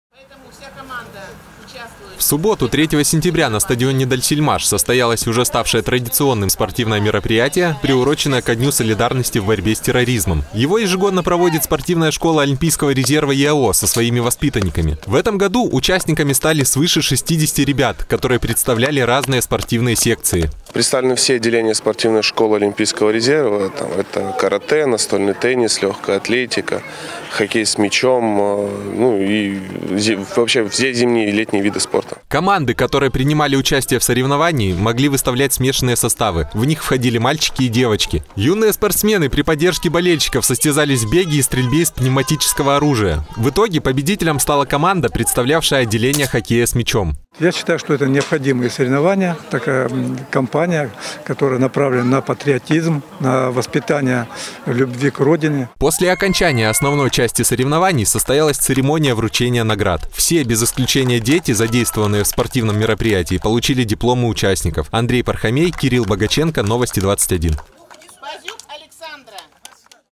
Спортсмены школы олимпийского резерва ЕАО присоединились ко Дню солидарности в борьбе с терроризмом
В субботу 3 сентября на стадионе «Дальсельмаш» состоялось уже ставшее традиционным спортивное мероприятие, приуроченное ко Дню солидарности в борьбе с терроризмом. Его ежегодно проводит Спортивная школа Олимпийского резерва ЕАО со своими воспитанниками. В этом году участниками стали свыше 60 ребят, которые представляли разные спортивные секции.